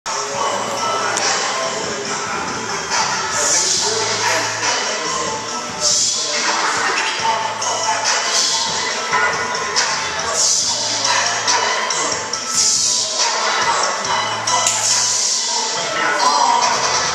Kris Jenkins Football Training at Athletes Edge
Kris Jenkins of the New York Jets warms up by squatting 315 lbs during football training camp at Athletes Edge.